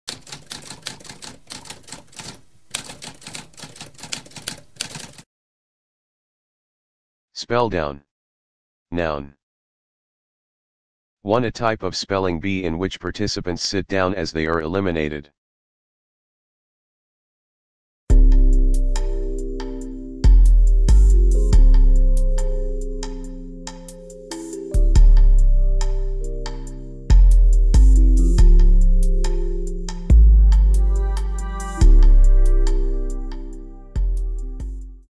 What does spelldown mean?
A spoken definition of spelldown.

Intro Sound:
Typewriter - Tamskp